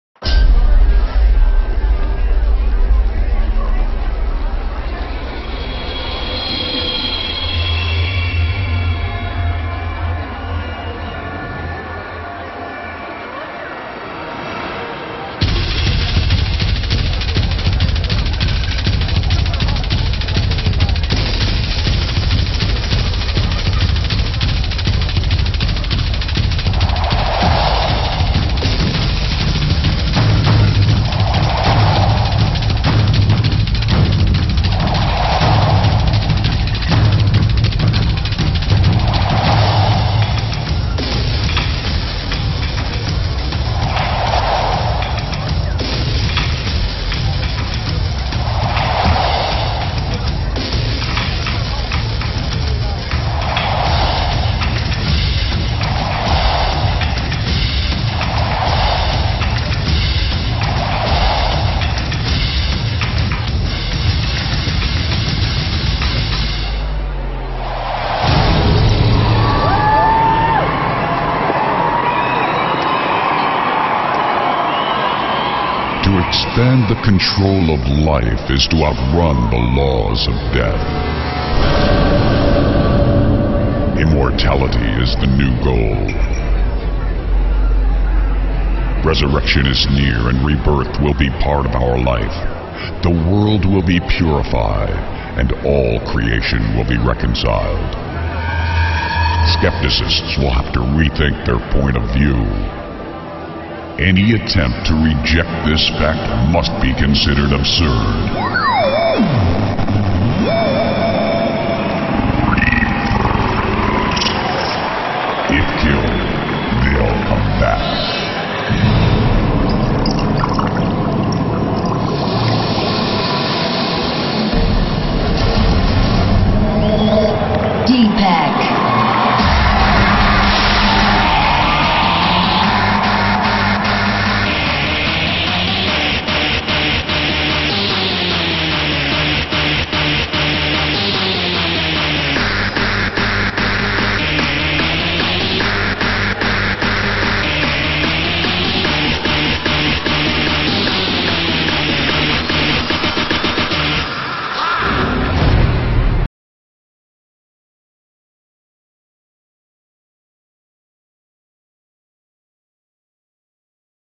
WICREATIONS 2009 Reverze Creation of Life
Reverze, the biggest and most famous hardcore dance event in Belgium introduced Creation of Life on 31 January 2009, a truly spectacular dance show. The idea was to have every DJ experience rebirth. For this purpose, WIcreations built a special cocoon (a sort of aluminum womb) from which each DJ emerged and presented himself to the wildly enthusiastic audience.

The cabin and control system in which the DJ (in the dark and thus invisible to the spectators) was transported to his cocoon was developed and built by WIcreations. By the way, all the DJs came into the world safe and sound.